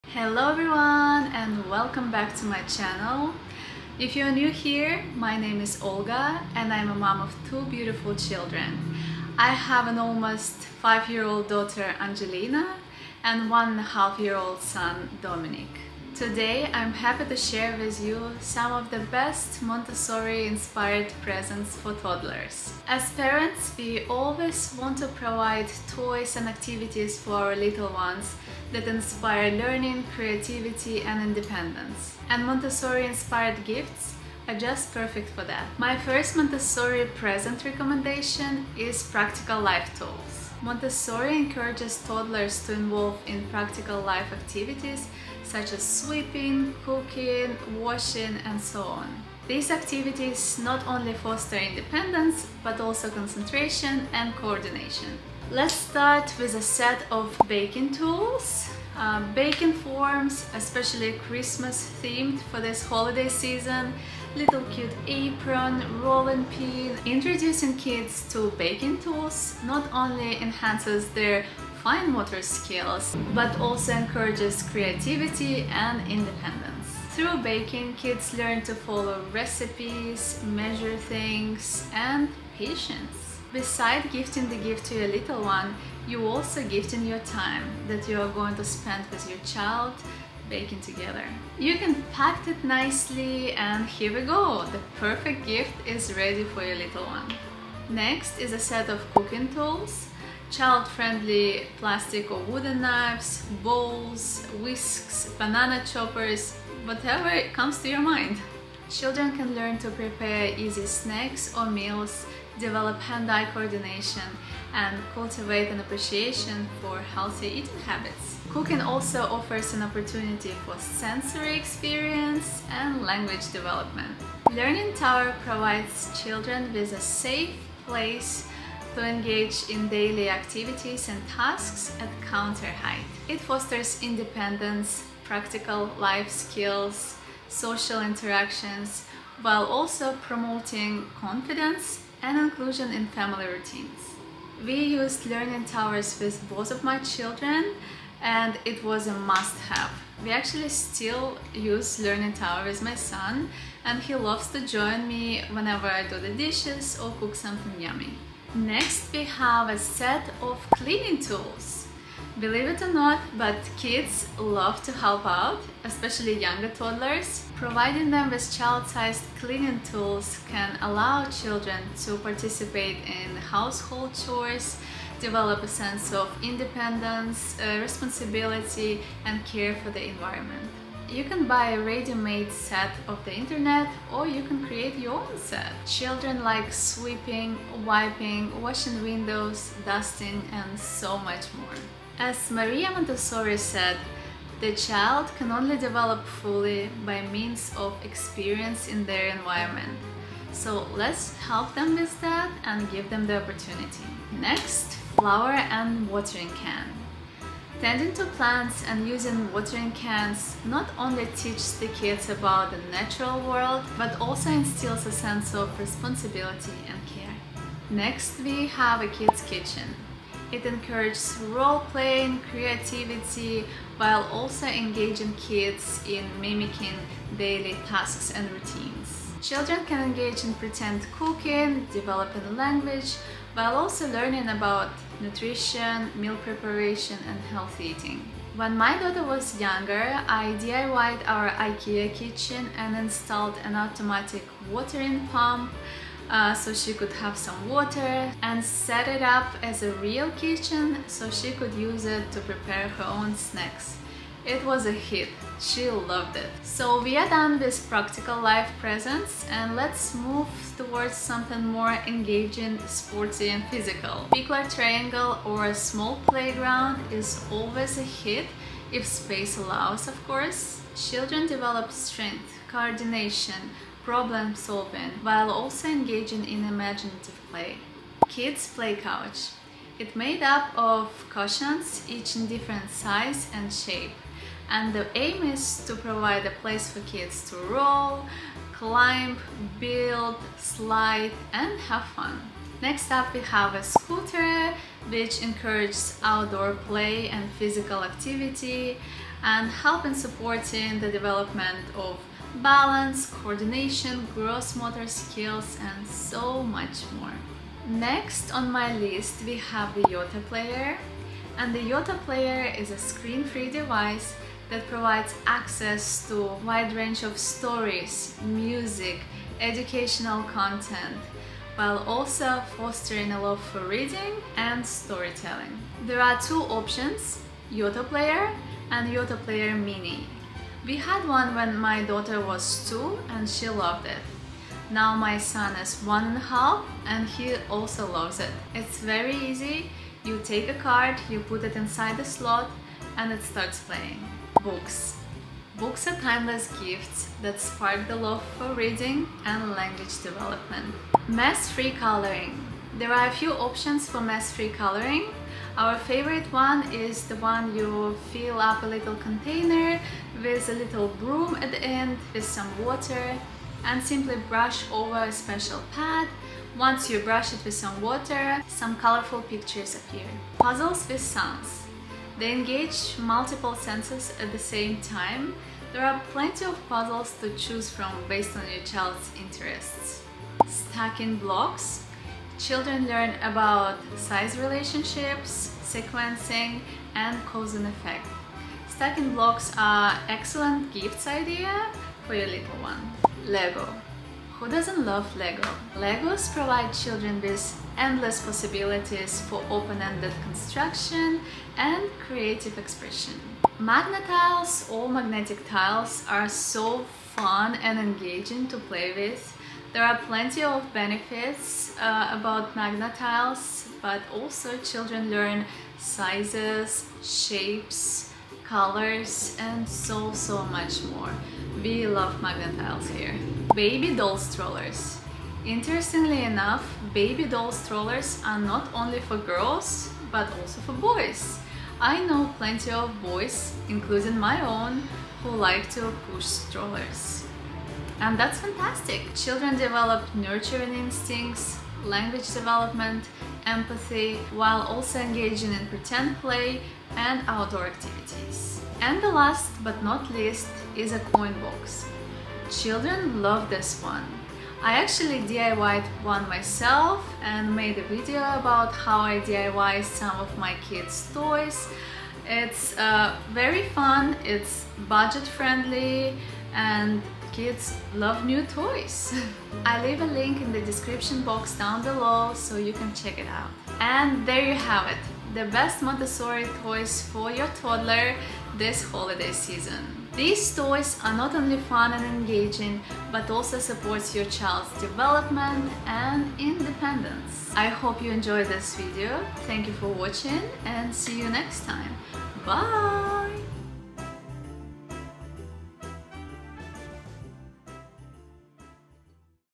Montessori & Waldorf Toddler Christmas Gift Guide 2024 | Educational toys that inspire independence
Welcome to the Ultimate Montessori Toddler Christmas Gift Guide 2024! Discover a selection of educational toys and Montessori-inspired gifts to inspire joyful learning, independence and creativity this holiday season. Find the perfect presents for the little ones in your life while embracing the principles of Montessori and Waldorf education. Join me in making this Christmas both meaningful and fun for your toddlers!

DON’T FORGET…
♡LIKE this video to let us know you enjoyed it!

0:00 Intro
0:36 Practical life gifts
4:21 Sports gifts
5:09 Yoto Player
5:45 Other gifts
7:50 Conclusion